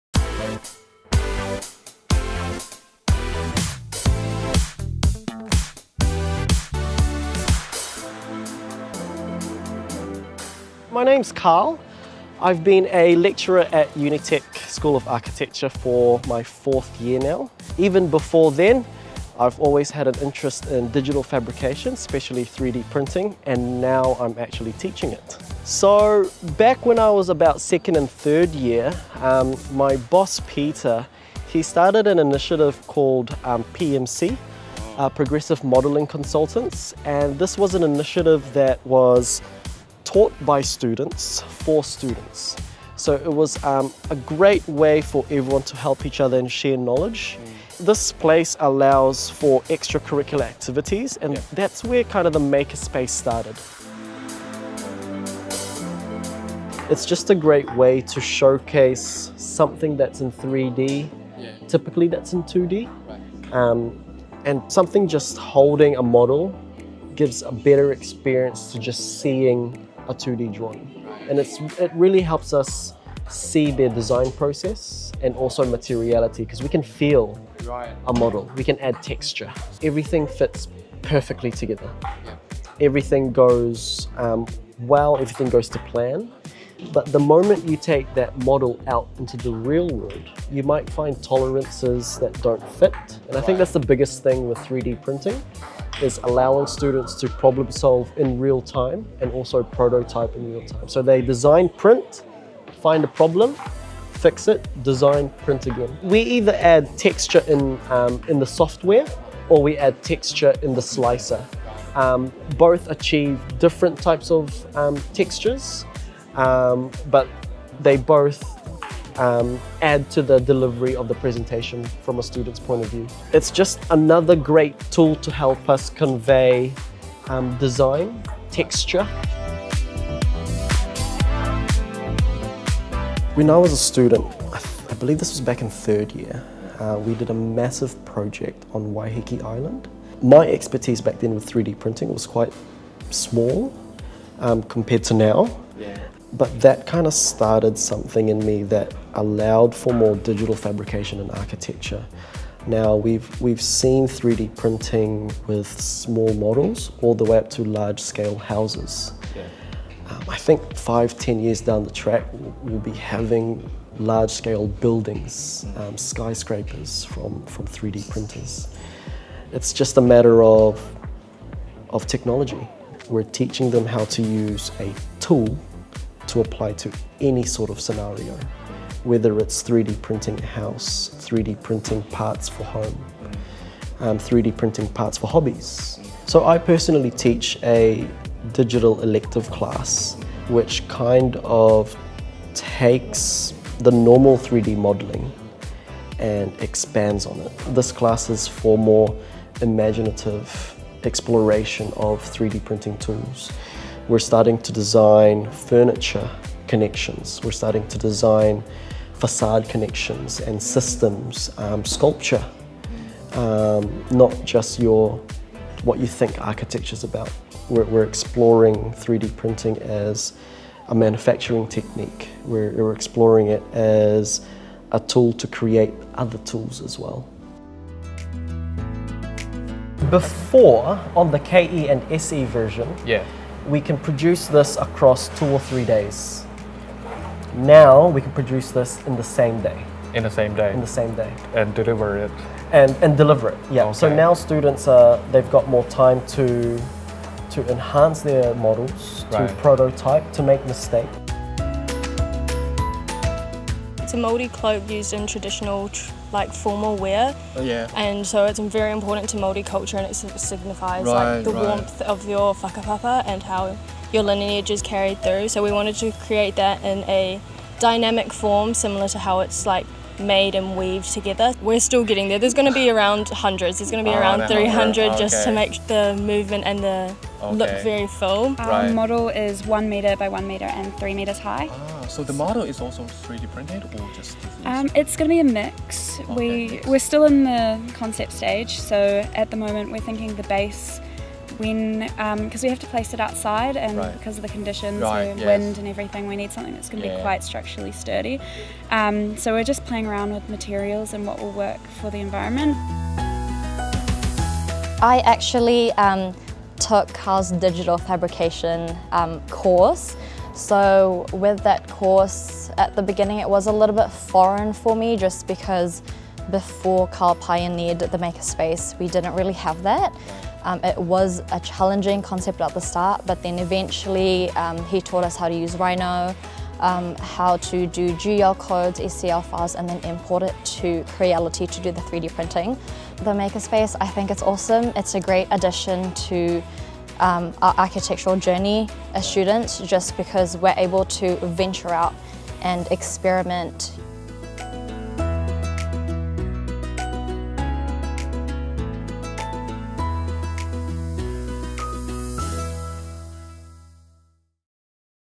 Creality Maker Spotlight| Sketch. Print. Build: How 3D Printing Is Changing Architecture Learning
🎥 Blueprints used to start on paper. Now? We start with 3D printers.
Welcome to MakerSpotlight, where 3D printing is changing how we design and build. 🌍🏗️

At Unitec in New Zealand @unitecnz  , architecture lecturer Carl Salas is re-defining how one learns architecture.
There, what started with just two desktop printers has grown into a full-fledged course and a hands-on Makerspace, where future architectures don’t just sketch out ideas — they 3D-print, test, build and create. 🎓

For Carl and all the students there, 3D printing isn’t just a tool.
It’s part of the design mindset — turning sketches into tangible designs all smooth and fast.

With full-scale 3D-printed houses already standing around the world, this shift in education is catching up with where the industry is heading. 🏠✨
3D printing's potential in architecture? Just limitless.

🌍 Maker Spotlight highlights inspiring makers and projects on 3D printing worldwide.